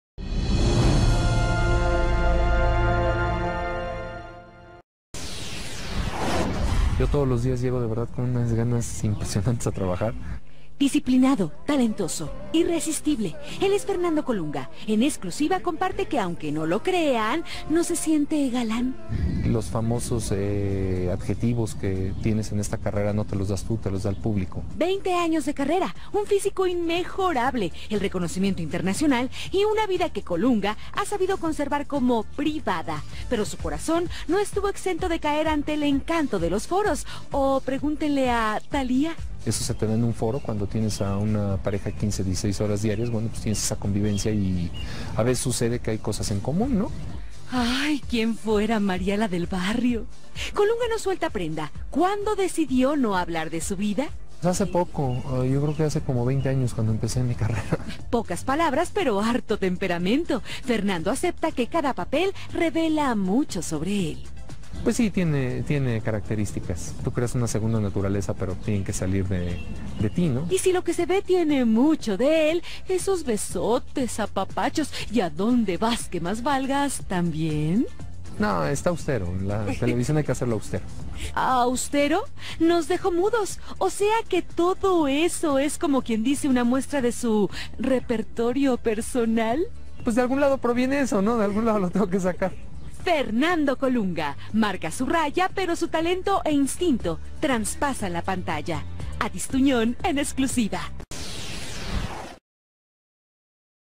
Fernando Colunga reconoce que sus personajes de las telenovelas tienen mucho de él mismo
Entrevista  a Fernando Colunga  cuándo estaba grabando la Telenovela "Mañana Es Para Siempre"  y ante las preguntas de la periodista  acepta que lo que se ve en pantalla en las escenas de amor ,  refleja su verdadera forma de ser .